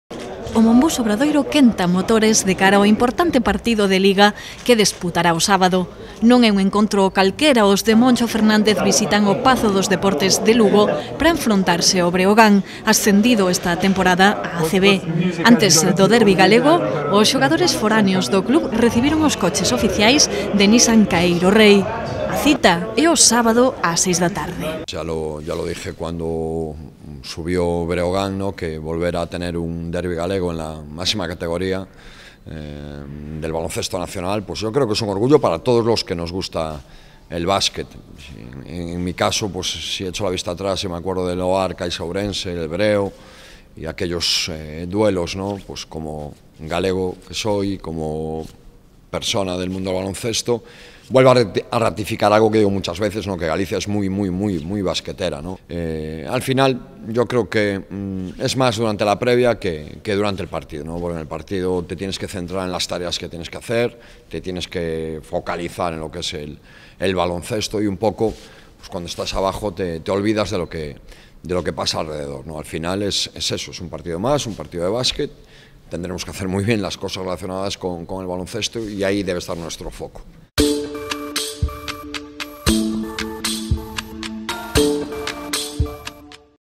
O Obradoiro quenta motores para o derbi co Breogán
O Monbus Obradoiro quenta motores de cara ao importante partido de Liga que desputará o sábado. Non é un encontro calquera, os de Moncho Fernández visitan o Pazo dos deportes de Lugo para enfrontarse ao Breogán, ascendido esta temporada á ACB. Antes do derbi galego os xogadores foráneos do club recibiron os coches oficioas de Nissan Caeiro Rey.

A cita é o sábado ás 6 da tarde.